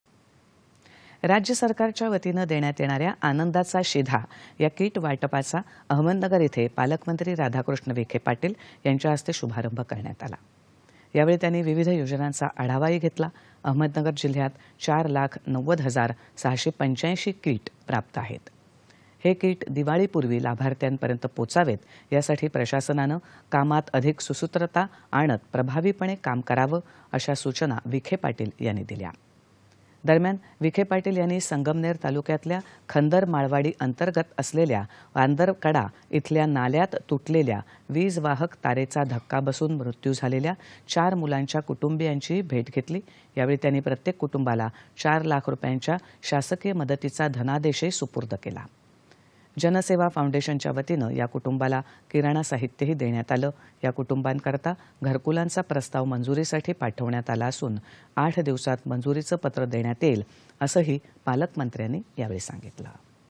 ‘आनंदाचा शिधा’ या कीट वाटपाचा अहमदनगर इथं पालकमंत्री राधाकृष्ण विखे-पाटील यांच्या हस्ते शुभारंभ
दि.२३.१०.२०२२ :
 राज्य सरकारच्या वतीनं देण्यात येणाऱ्या ‘आनंदाचा शिधा’ या कीट वाटपाचा अहमदनगर इथं पालकमंत्री राधाकृष्ण विखे-पाटील यांच्या हस्ते शुभारंभ करण्यात आला.  यावेळी त्यांनी विविध योजनांचा  आढावा घेतला. अहमदनगर जिल्ह्यात ४ लाख ९० हजार ६८५ कीट प्राप्त आहेत. हे कीट दिवाळीपूर्वी लाभार्थ्यांपर्यंत पोहोचावेत यासाठी प्रशासनानं कामात अधिक सुसूत्रता आणत प्रभावीपणे काम करावं अशा सूचनाही विखे पाटील यांनी दिल्या आहेत. दरम्यान, विखे पाटील यांनी संगमनेर तालुक्यातल्या खंदरमाळवाडी अंतर्गत असलेल्या वांदरकडा इथल्या नाल्यात तुटलेल्या वीजवाहक तारेचा धक्का बसून मृत्यू झालेल्या चार मुलांच्या कुटुंबीयांची भेट घेतली. यावेळी त्यांनी प्रत्येक कुटुंबाला चार लाख रुपयांच्या शासकीय मदतीचा धनादेश  सुपुर्द केला. जनसेवा फाउंडेशनच्या वतीनं या कुटुंबाला किराणा साहित्यही देण्यात आलं. या कुटुंबीयांकरिता घरकुलांचा प्रस्ताव मंजूरीसाठी पाठवण्यात आला असून आठ दिवसात मंजुरीचं पत्र देण्यात येईल.असं पालकमंत्र्यांनी सांगितलं.